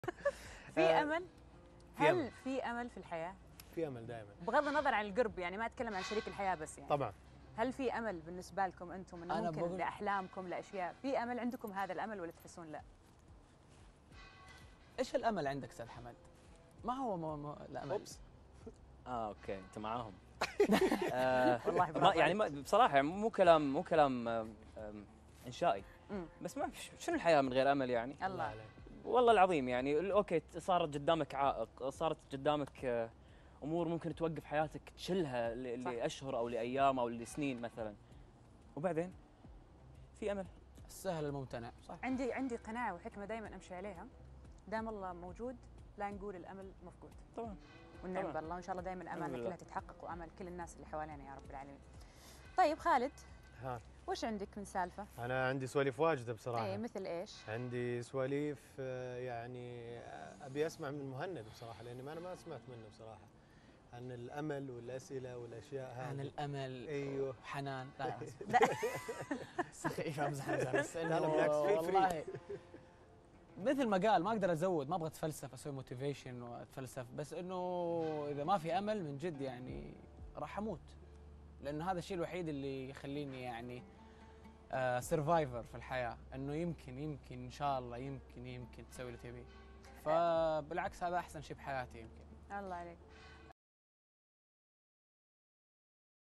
حمد أشكناني: الحياة ليس لها معنى بدون أمل وهذا كلام ليس إنشائي
حمد أشكناني: الحياة ليس لها معنى بدون أمل وهذا كلام ليس إنشائي.. ومهند الحمدي: بدون فلسفة إذا ما في أمل لن أقدر على الاستمرار في الحياة